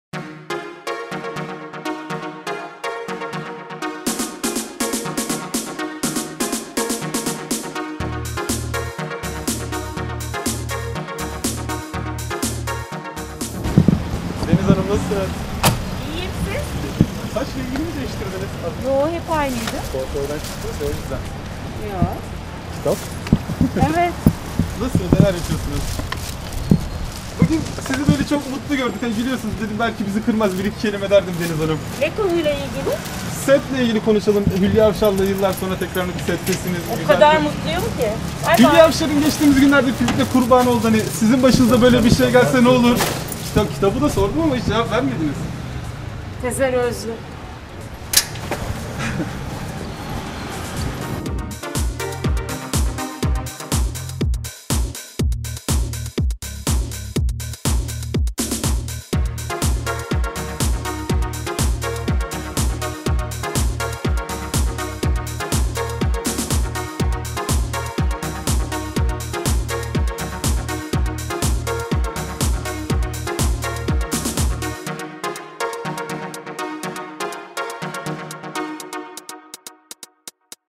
Deniz Çakır'ın Yıllar Sonra Gelen Hülya Avşar Mutluluğu | HEP AYNIYDI
Masumiyet dizisindeki performansı ile hayran bırakan Deniz Çakır, önceki gün Kuruçeşme’deki evine girerken magazinburada.net kameralarına yansıdı.

Kuaförden çıktığını ve saç rengini değiştirip değiştirmediği sorulan oyuncu, “Hayır, hep aynıydı” şeklinde konuştu.

Ekranların sevilen dizisi Masumiyet ile ilgili soruların yöneltildiği Deniz Çakır, yıllar sonra Hülya Avşar ile aynı sette yer alması hakkında “O kadar mutluyum ki…” demekle yetindi.

Dizide Hülya Avşar’ın canlandırdığı karakterin başına gelenlerin kendi başına gelmesi halindeki tepkisi de sorulan Çakır, sessiz kalmayı tercih etti. Sonrasında ise elindeki, Leyla Erbil’in ‘Tezer Özlü'den Leyla Erbil'e Mektup’ adlı kitabını gösterip evine girdi.

Hülya Avşar ve Deniz Çakır, 2005 yılında ‘Kadın İsterse’ dizisinde birlikte rol almıştı. İki isim, 16 yıl sonra Masumiyet dizisi ile birlikte yeniden birlikte çalışmaya başladı.